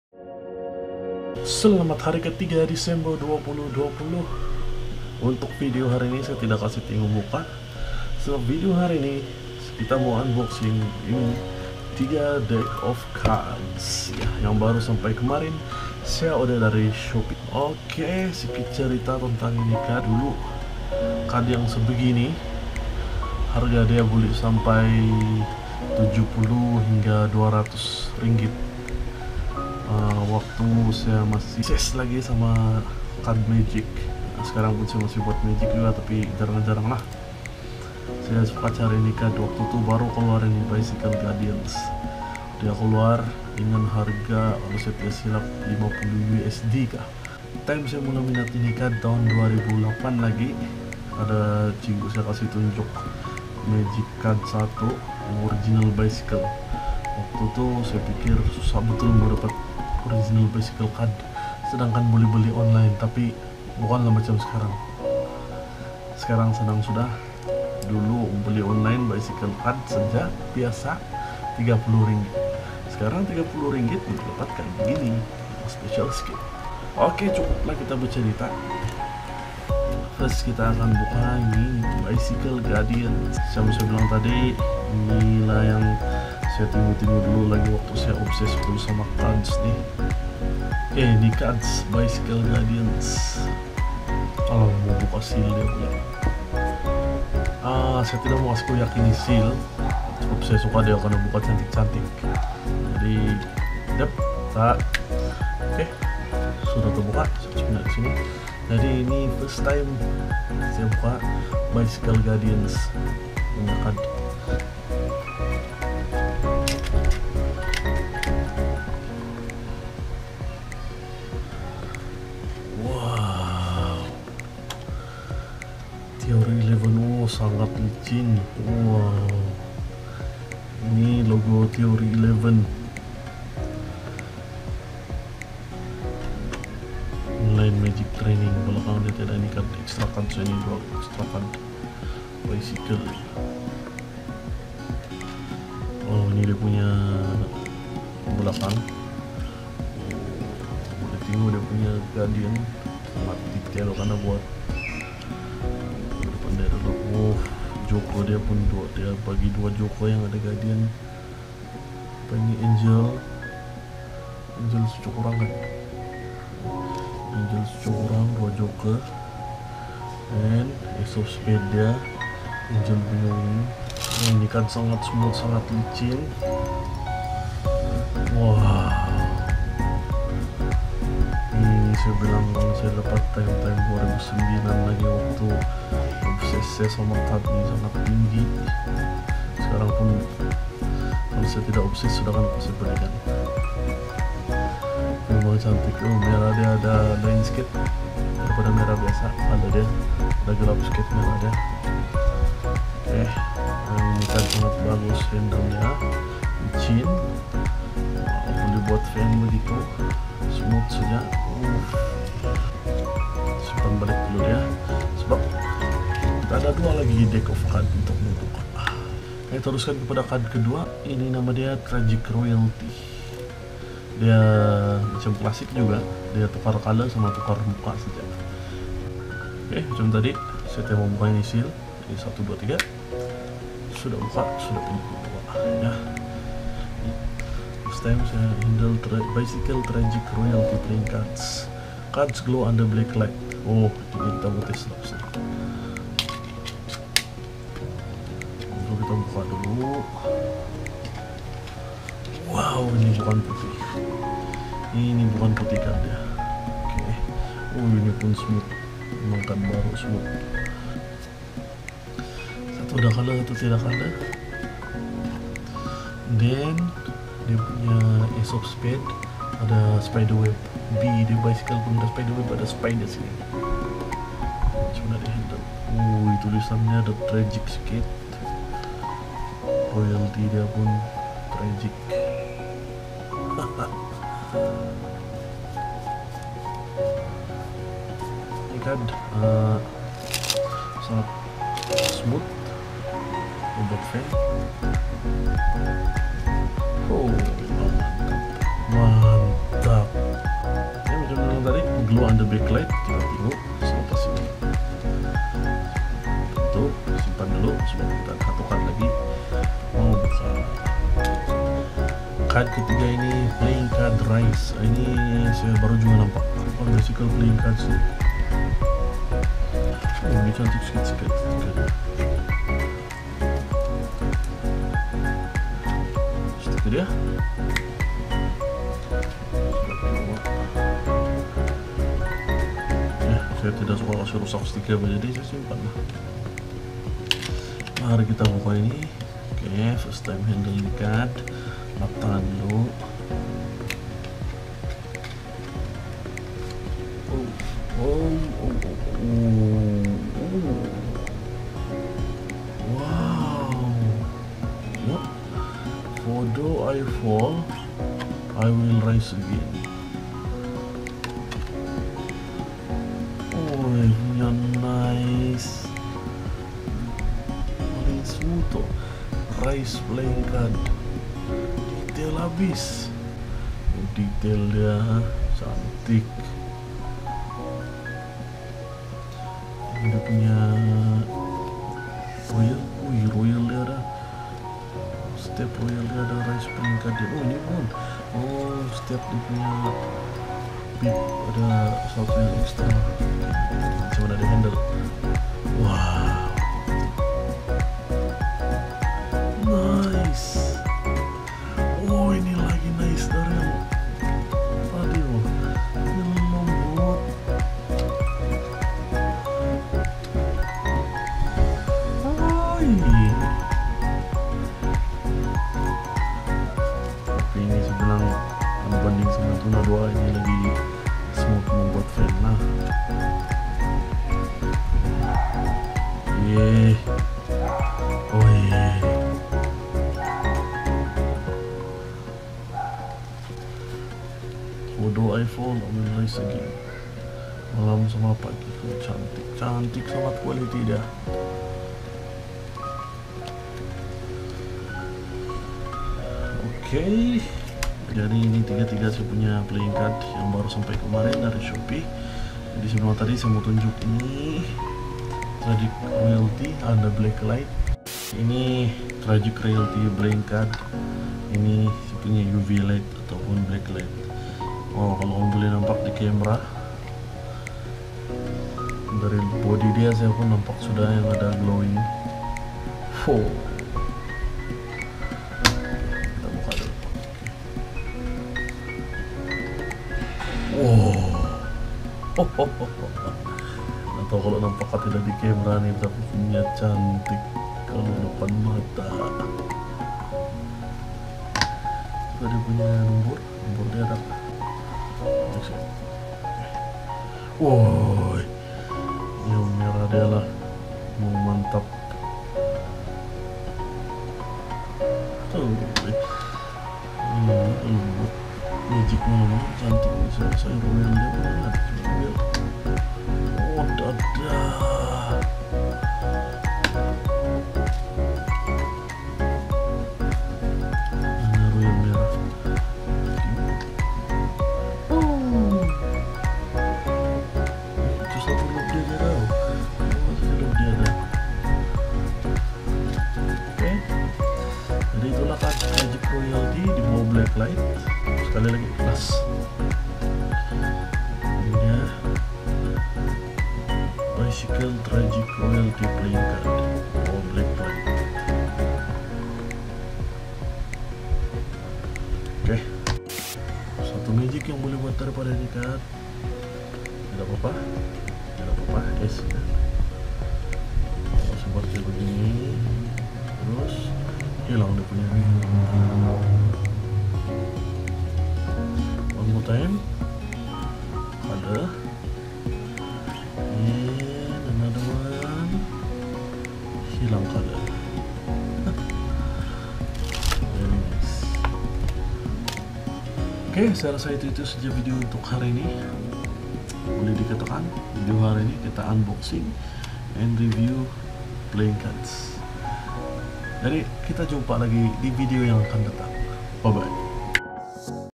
03 UNBOXING AND REVIEW PLAYING CARDS
bah jadi kemarin sia sampai suda kad2 magic sia dri shopee.. jadi niari sia buka depan camera trus review2 knun ni..